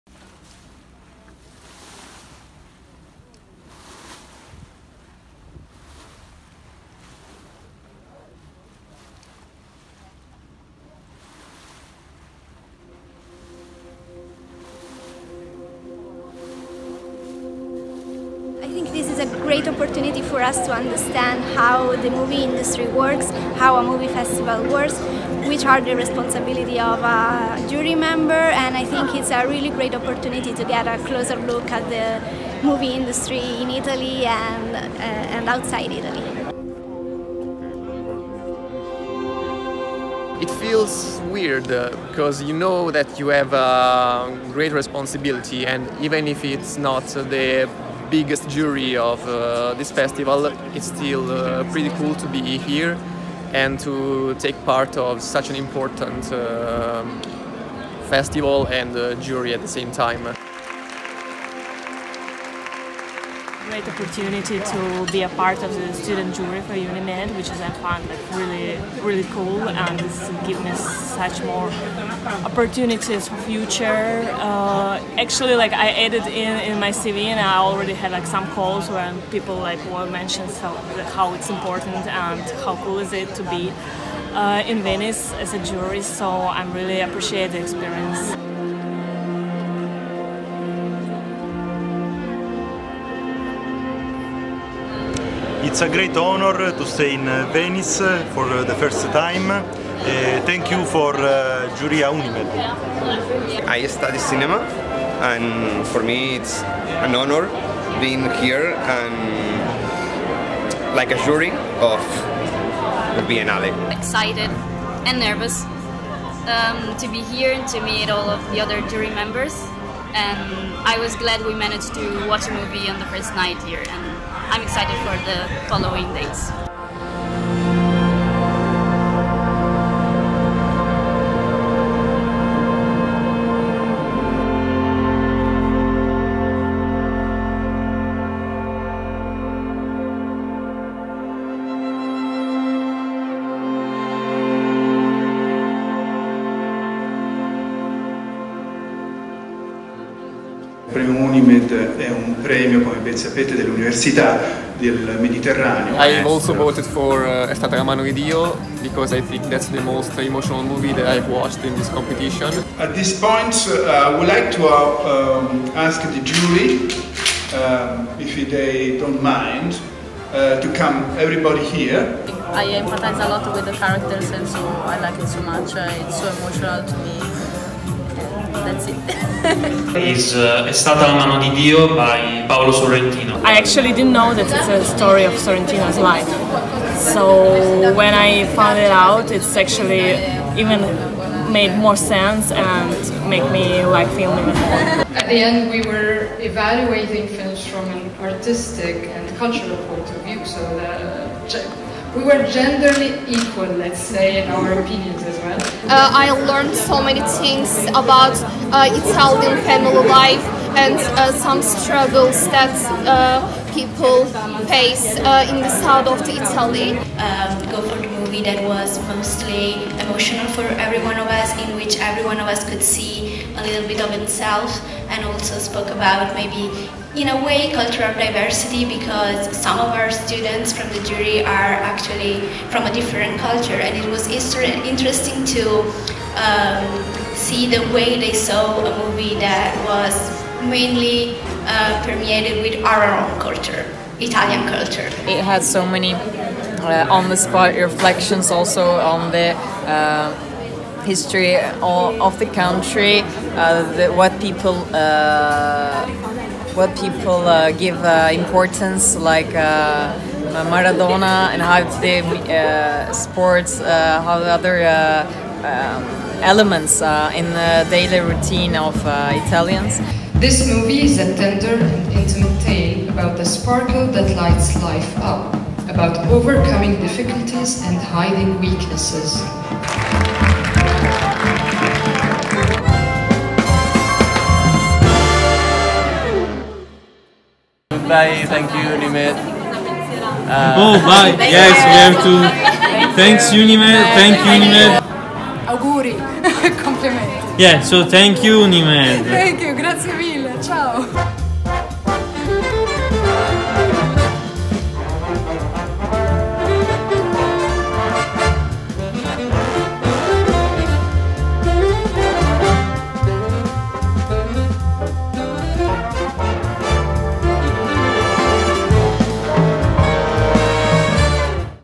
UNIMED Award 2021
The UNIMED Award, collateral prize of the Venice International Film Festival, results from the collaboration between UNIMED and la Biennale of Venice – one of the most prestigious international film festivals worldwide.
A jury of about 15 international students coming from UNIMED associated universities evaluates each year the competing movies and assigns the prize to the best one.